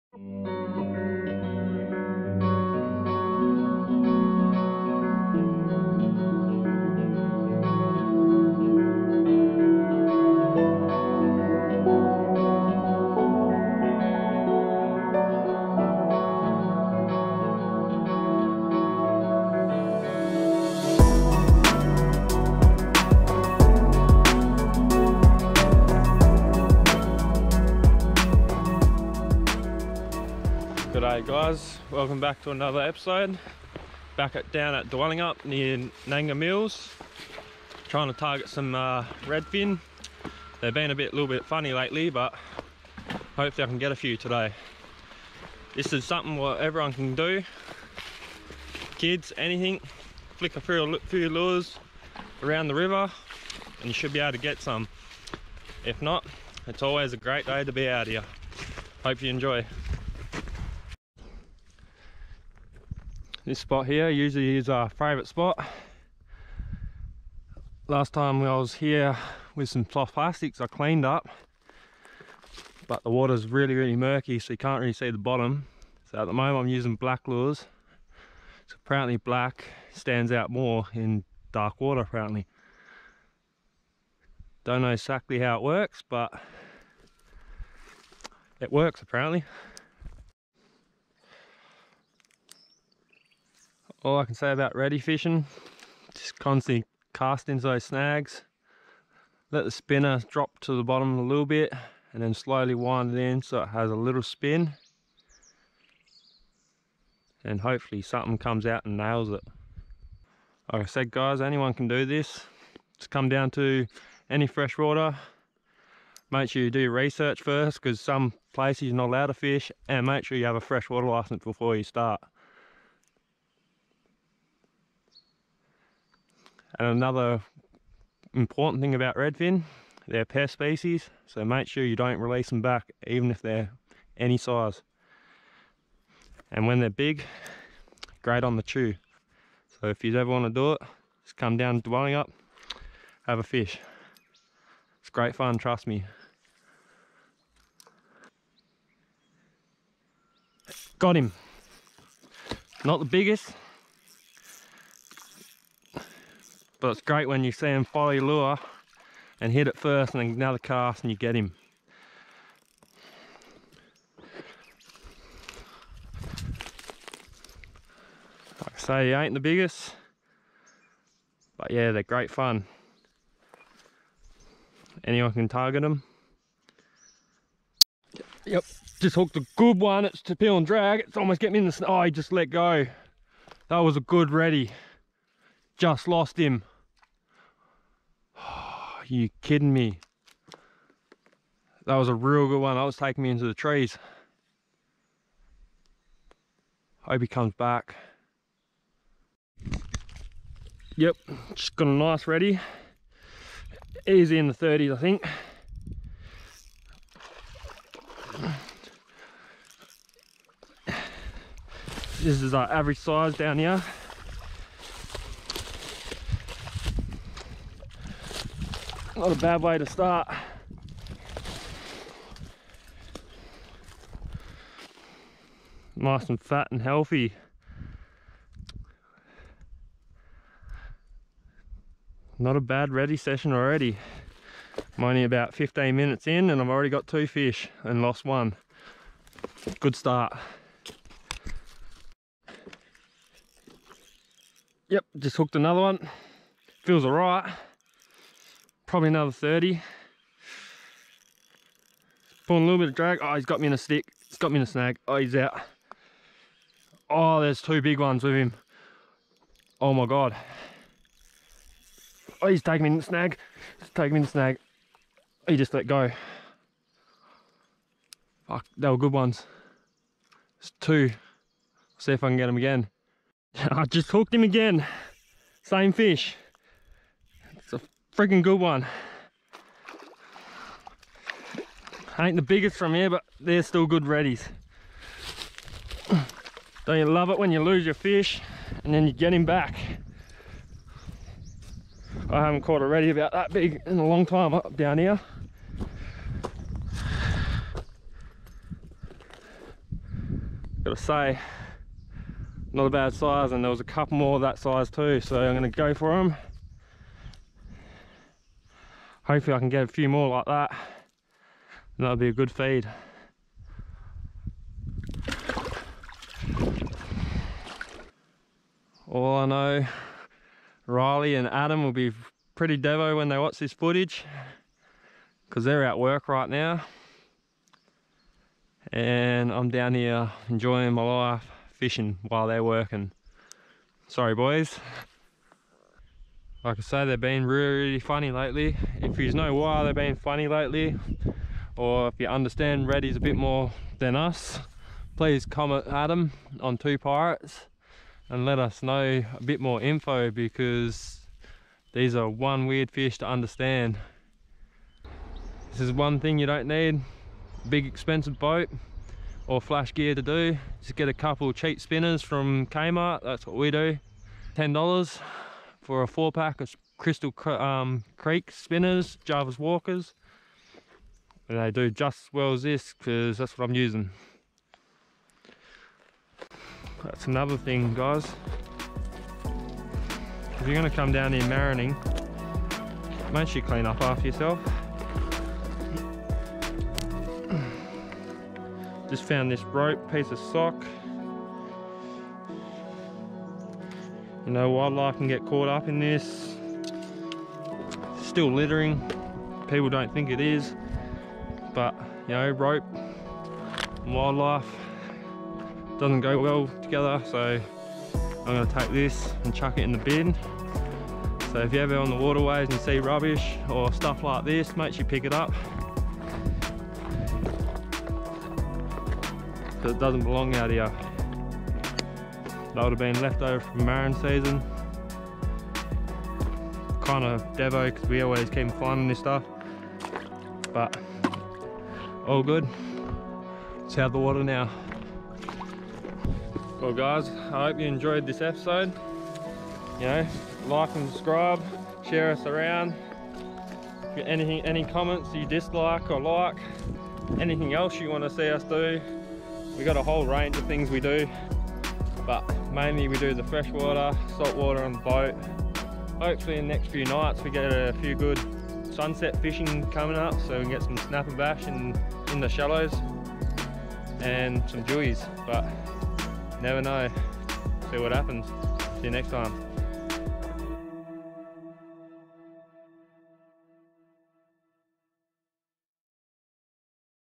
Solo Redfin perch hunt in dwellingup western Australia (yellow Perch)
Reed headed out to Nanga on a solo redfin hunt.
even with a bad back, he shows just how easy it is for the whole family to get out and chase redfin perch and trout in WA's Southwest.